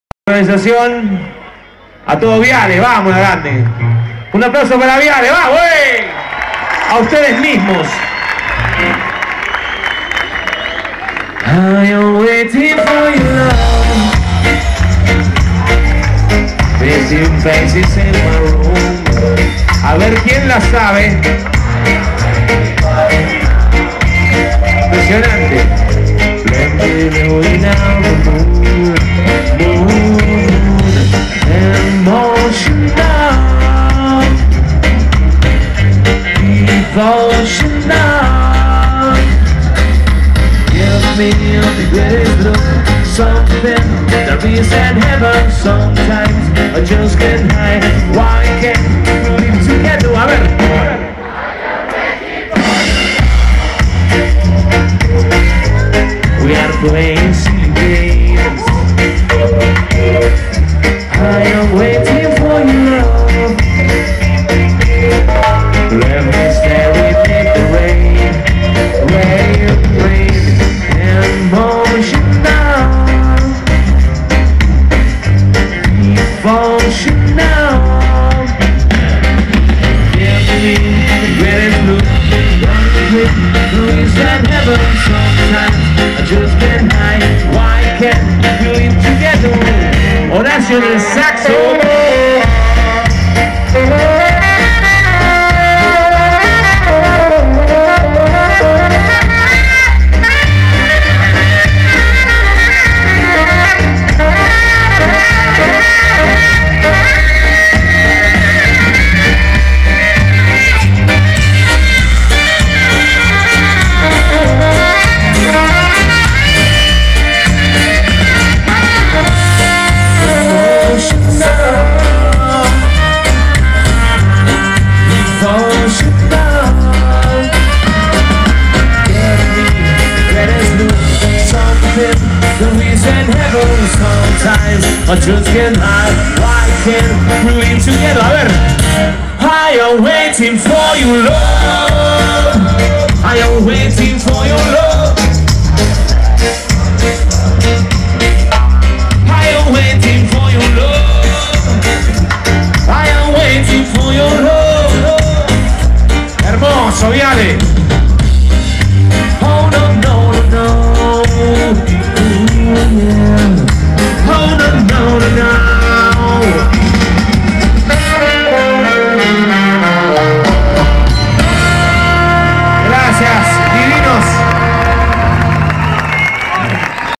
Los Pericos en la Fiesta del Asado con Cuero 2012 1
Concurso de Asadores
Stands comerciales.
Noche de domingo:
No hay grama
12 Monos
Entrega de premios Concurso Nacional del Asado con Cuero
Nomeleskatime
y Los Pericos